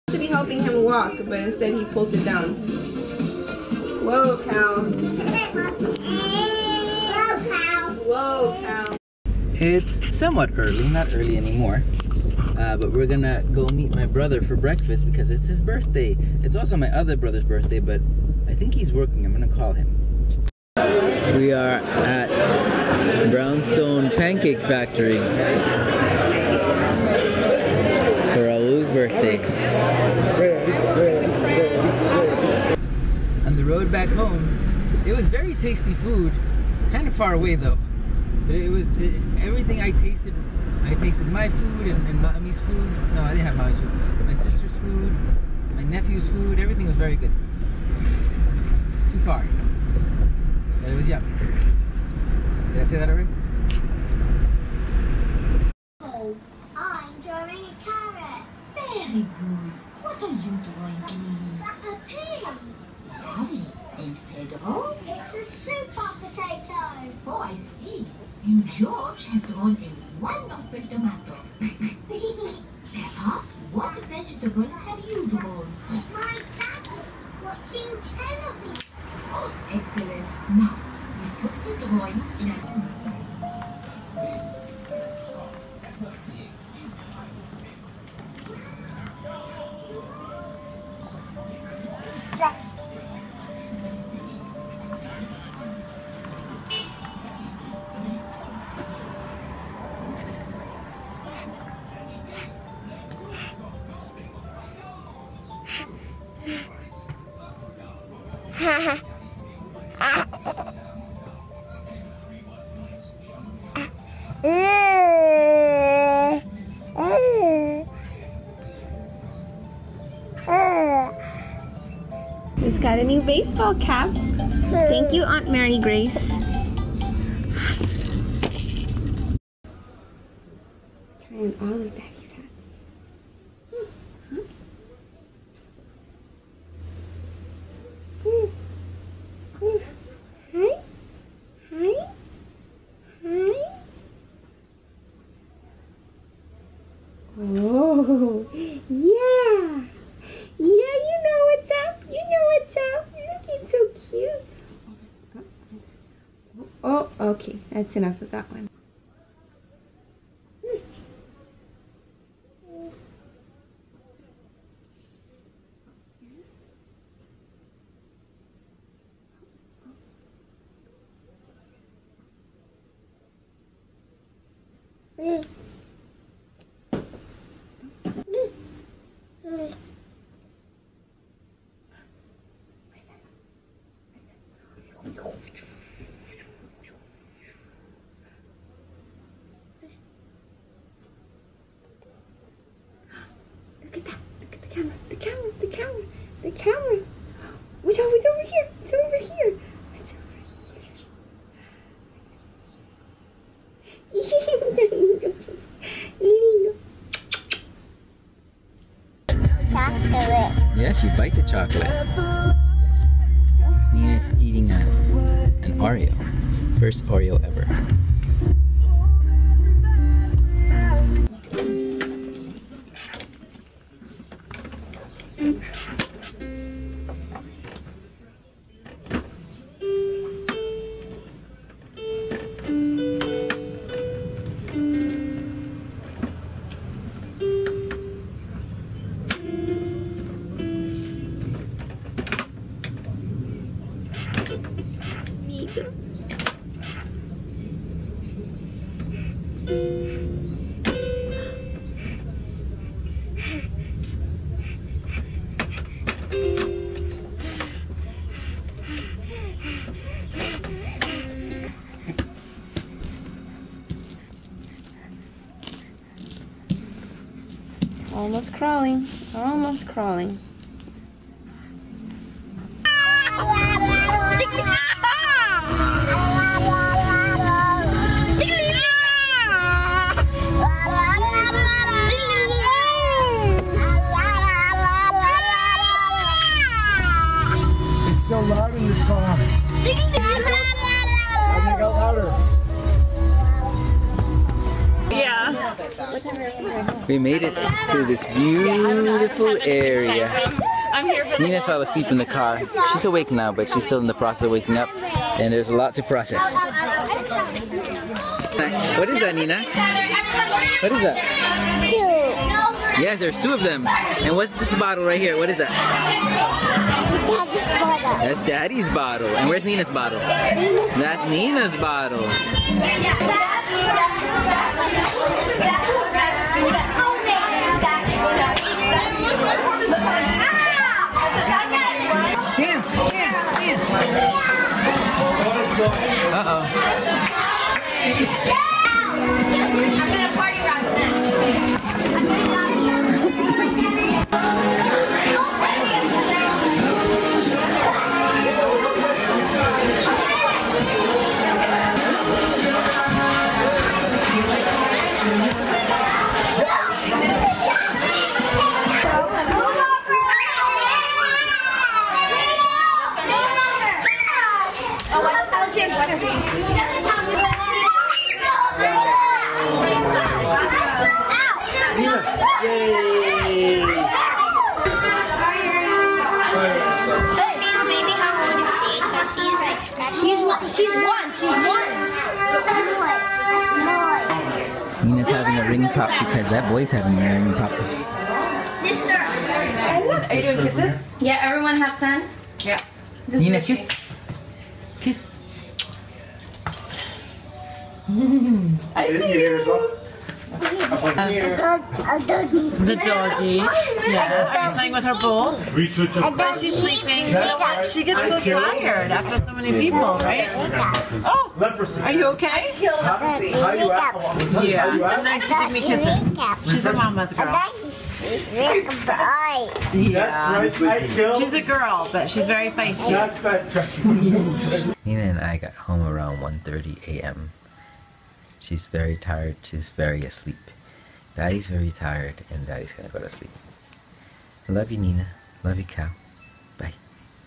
Day 216, Brownstone, Makeup, Hats, and a Party
I spent a few hours on the road but we still had a nice day